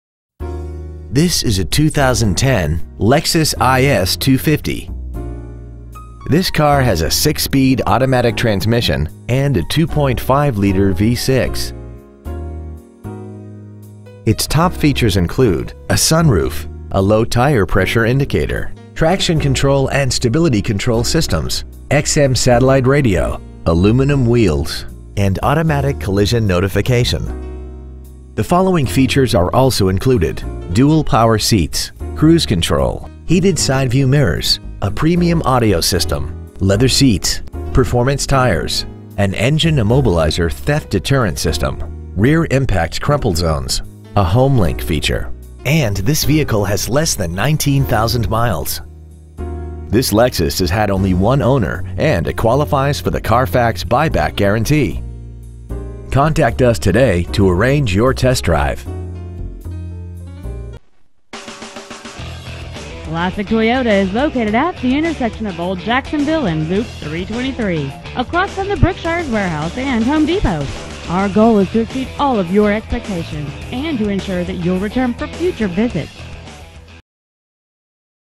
2010 Lexus IS 250 Tyler Texas 75701
Year : 2010
 Make : Lexus
 Model : IS 250
 Engine : Gas V6 2.5L/152
 Trans . : Automatic
 Exterior : White
 Miles : 18,816

 1717 West Sw Loop 323

 Preowned 2010 Tyler Texas